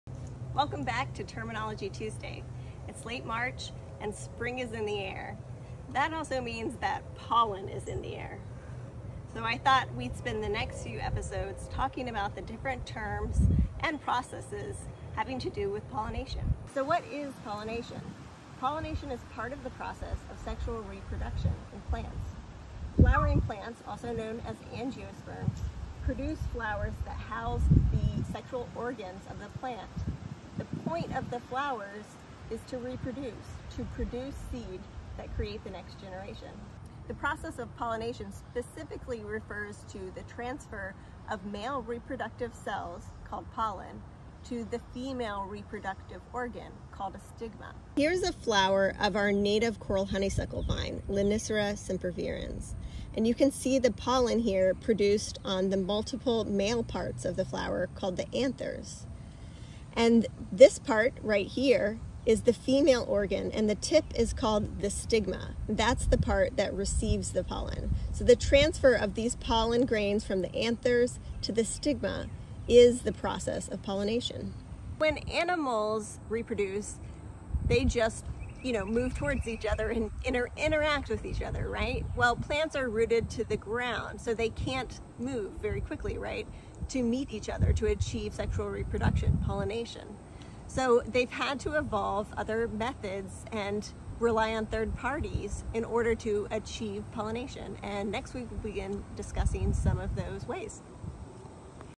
What IS Pollination?! 🌿
TERMINOLOGY TUESDAY 🌿
w/ the Florida Native Plant Society!

It’s late March, and spring is in the air! That means pollen is in the air, too!

For the next few episodes, we will go over various terms and processes related to pollination.

Pollination is part of the process of sexual reproduction in plants. Flowering plants, aka angiosperms, produce flowers to reproduce; flowers house the plant's sexual organs. Plants produce flowers to achieve sexual reproduction and produce seeds - their offspring.

Pollination refers specifically to the transfer of the male reproductive cells (pollen) to the female reproductive organ (stigma). When pollination is achieved, the flower is fertilized and can produce seed - the next generation!

Because plants are mostly immobile, they cannot move toward each other to achieve pollination and must rely on third parties or other processes to ensure sexual reproduction.

Next week, we will go over some of the various methods plants have evolved to achieve pollination.